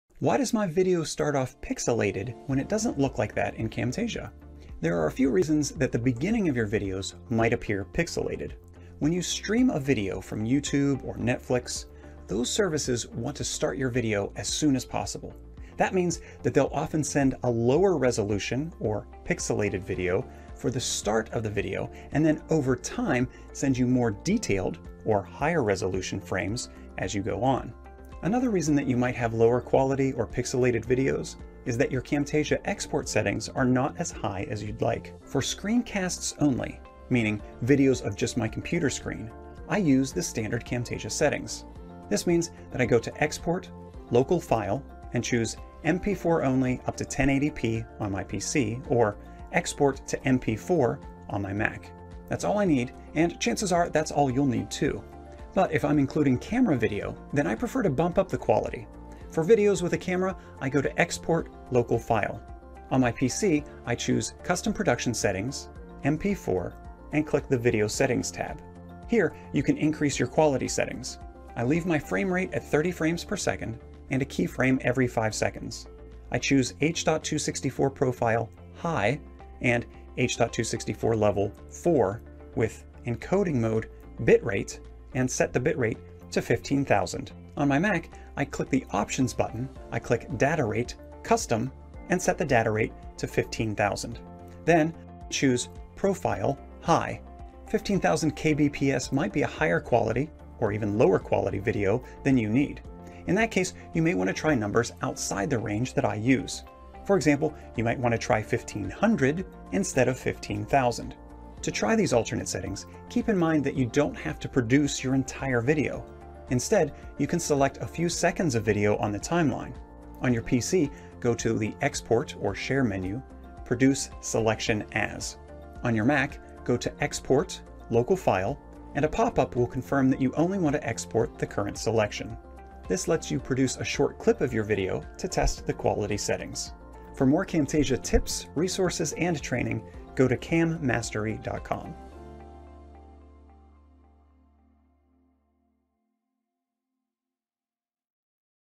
Pixelation and Export Settings in Camtasia
Why does my Camtasia video look pixelated? A few answers to today's question.